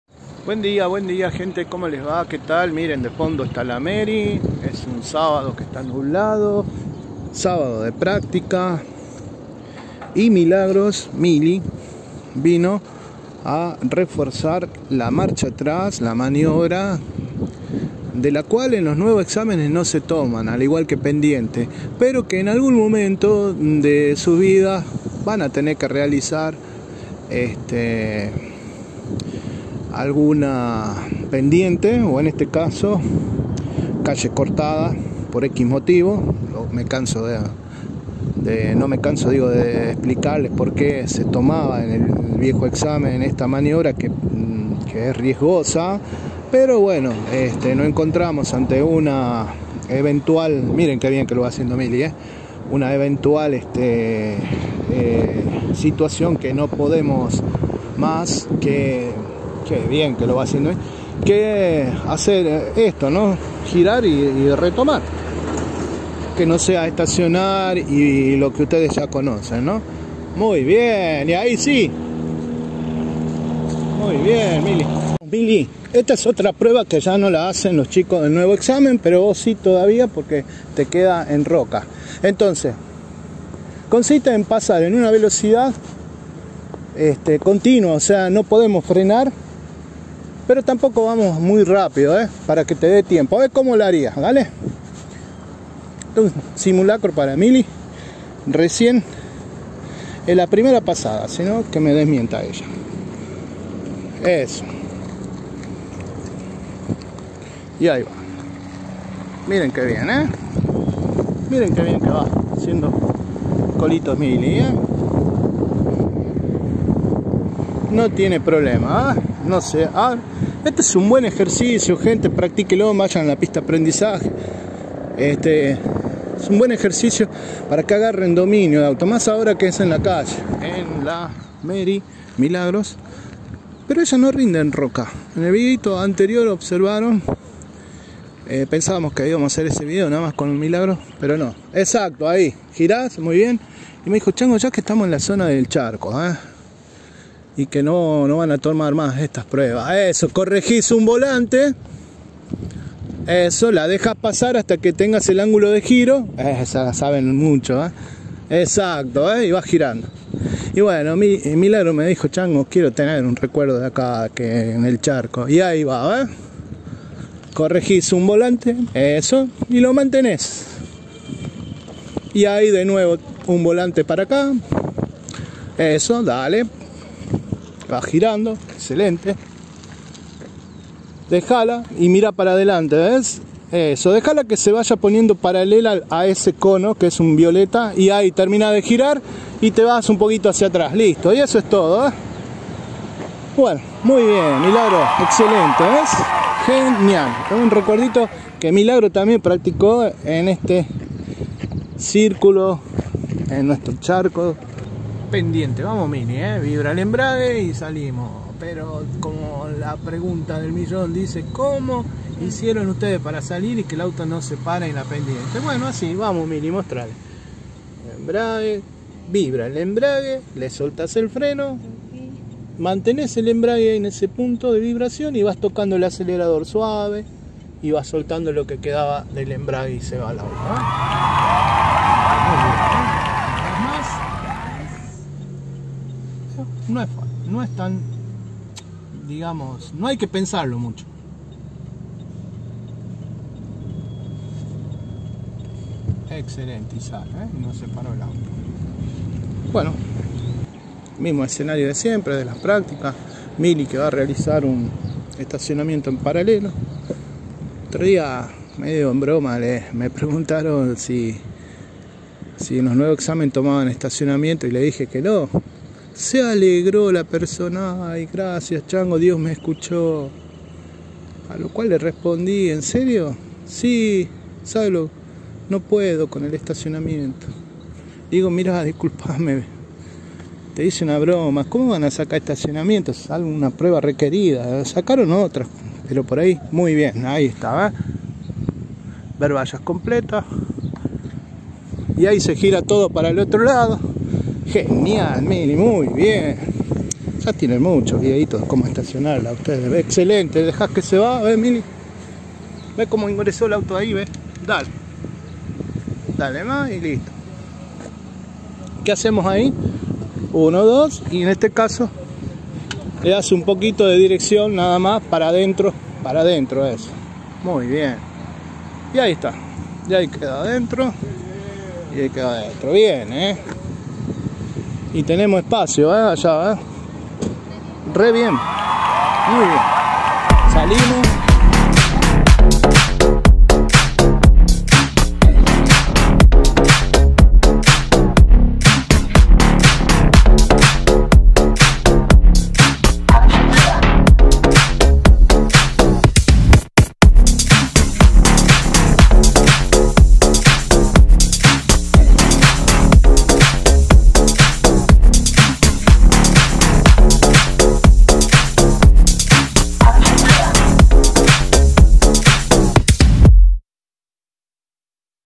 Mili : Clases prácticas de manejo en tránsito - Chango Autoescuela - Simulacro de exámen CABA.
Clases prácticas en tránsito. Clases de manejo en CABA  Chango Autoescuela Simulacro  Comunas exámen práctico. Nuevo exámen práctico de manejo para obtener la licencia de conducir en la ciudad de Buenos Aires.